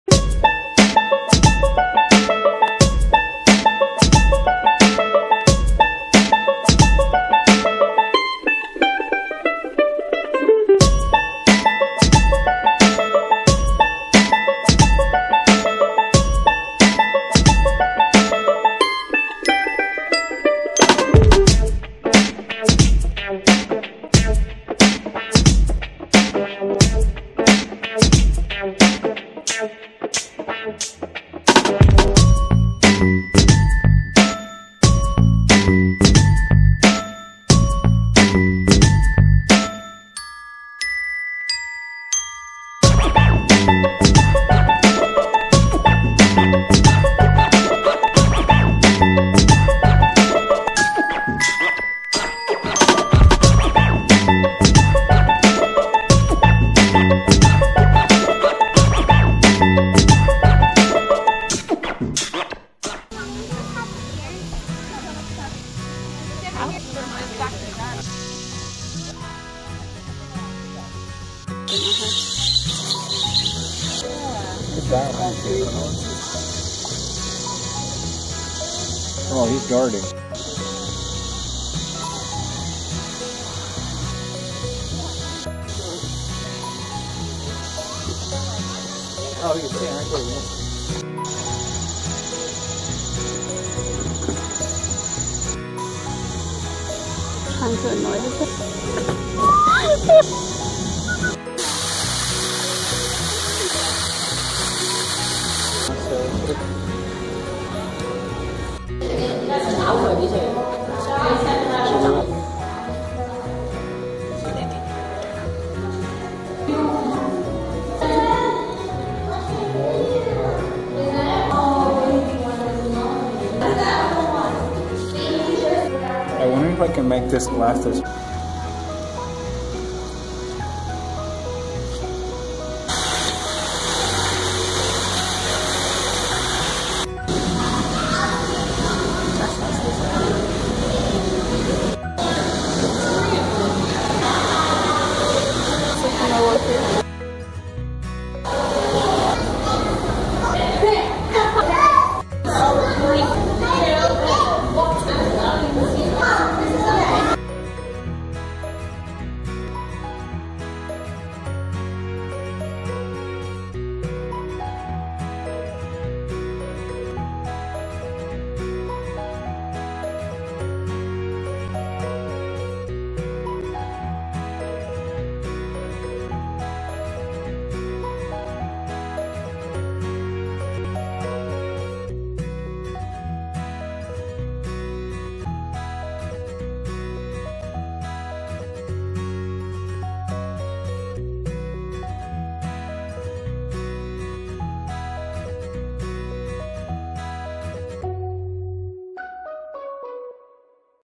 August 21, 2015 - Atlanta Zoo...!
Day two of birthday festivities, spent at the Atlanta Zoo...:)